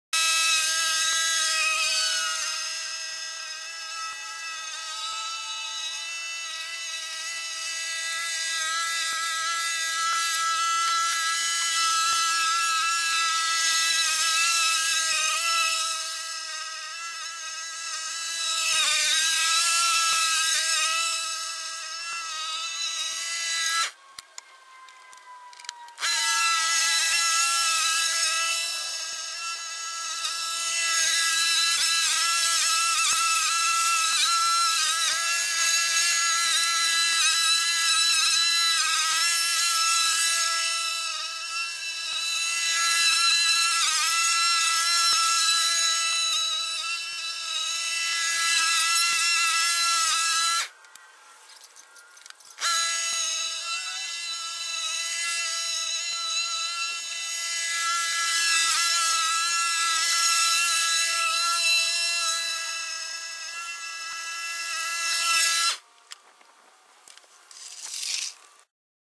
The Lawn Mower Man
High Speed lawn mowing.

14 minutes of work cut down to 1 minute.

The buzzing bee noise is the actual sound from the lawn mower.